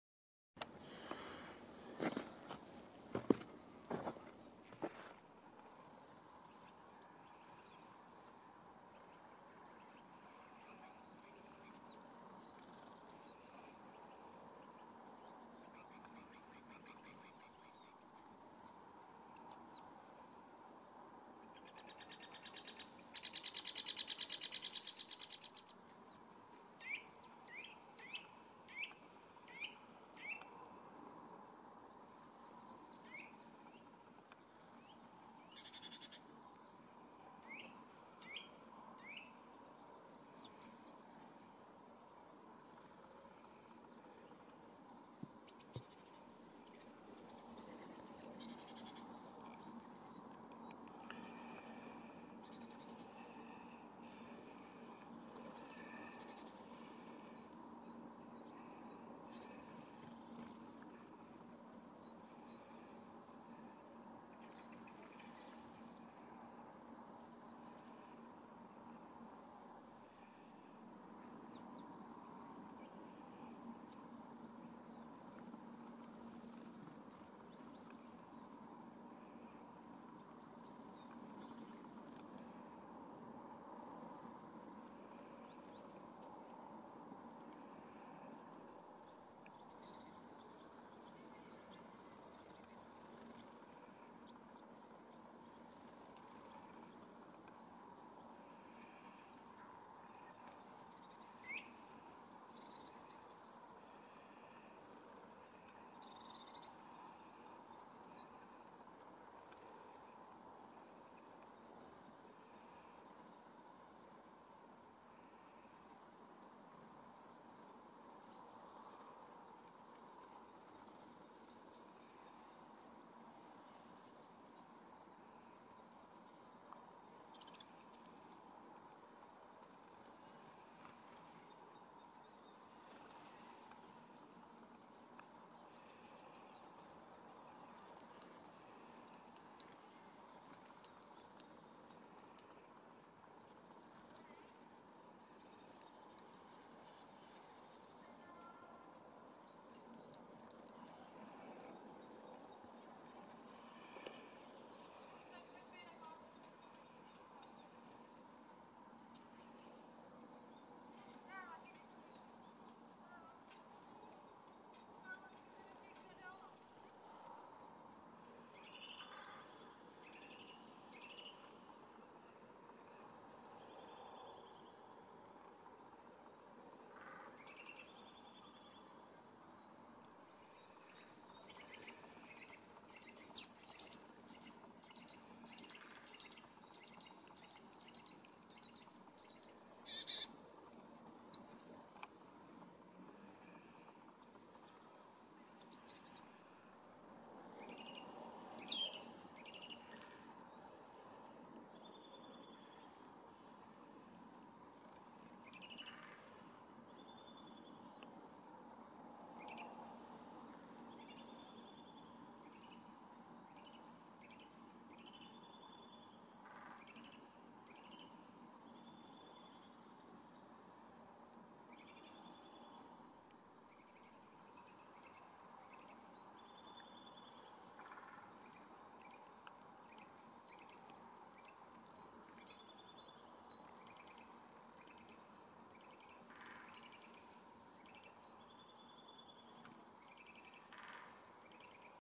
Lesní krmítko 24. 1. 2021
Strakapoud prostřední, sýkora babka, koňadra, brhlík lesní